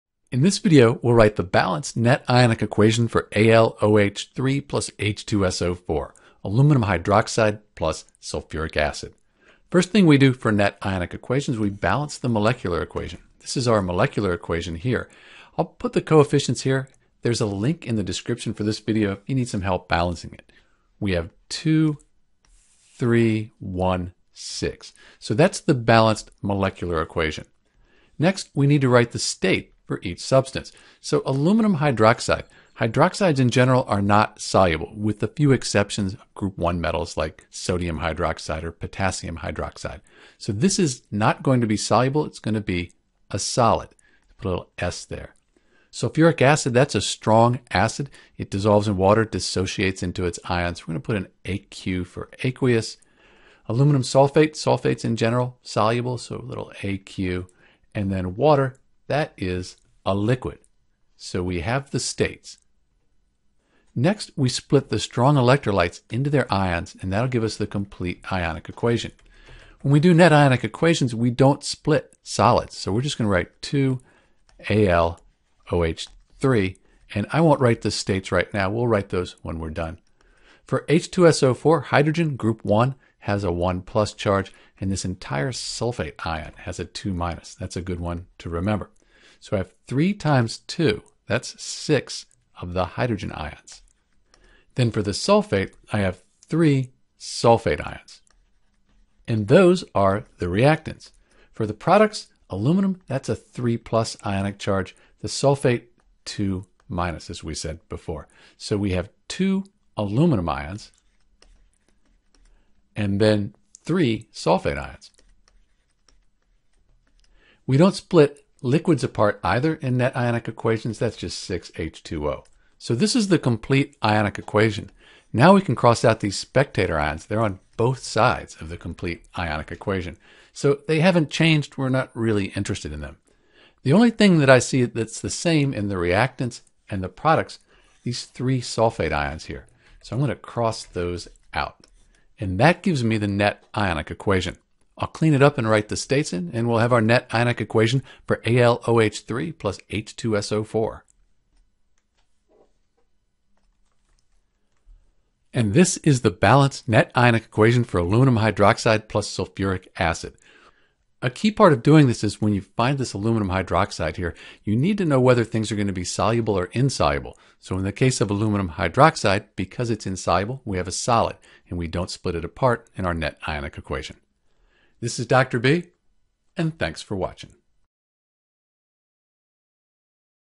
How to Write the Net Ionic Equation for Al(OH)3 + H2SO4 = Al2(SO4)3 + H2O
There are three main steps for writing the net ionic equation for Al(OH)3 + H2SO4 = Al2(SO4)3 + H2O (Aluminum hydroxide + Sulfuric acid). First, we balance the molecular equation.

Important Skills

More Practice

General Steps:

1. Write the balanced molecular equation.
2. Write the state (s, l, g, aq) for each substance.
3. Split soluble compounds into ions (the complete ionic equation).
4. Cross out the spectator ions on both sides of complete ionic equation.
5. Write the remaining substances as the net ionic equation.